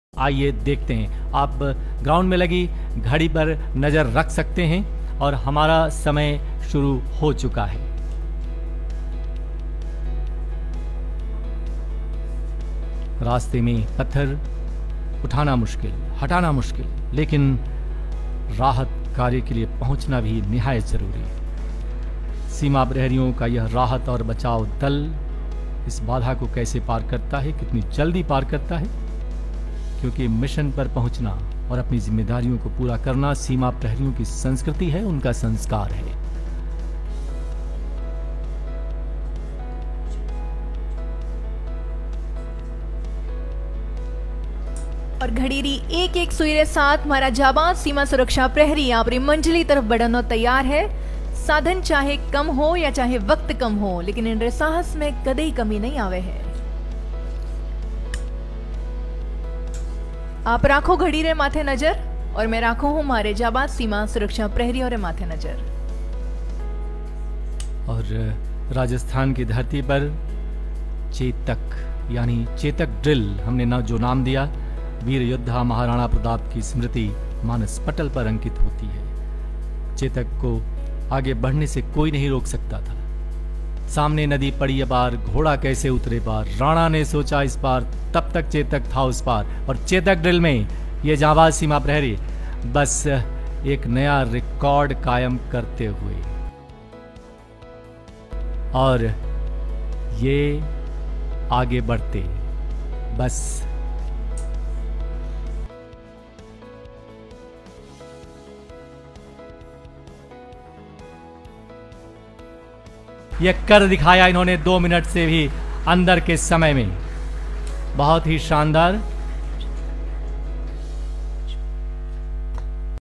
BSF के जवानों ने दिखाया गजब का करतब, दो मिनट में खोलकर तैयार की पूरी गाड़ी | Aaj Tak
दो मिनट में नूडल बने या न बने लेकिन, इस दो मिनट में BSF वालों ने गाड़ी खोलकर ज़रूर बंद कर दी, देखिए BSF के जवानों ने कैसे दिखाया गजब का करतब.

Video Courtesy: @BSF_India